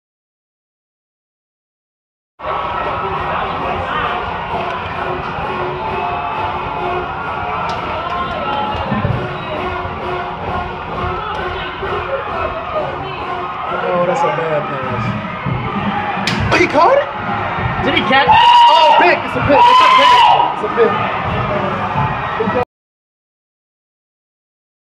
Interception preserves Higgins victory
Davion Sullivan, who caught the go-ahead score just seconds before, clinches Higgins&apos; first win of the season with an interception in the endzone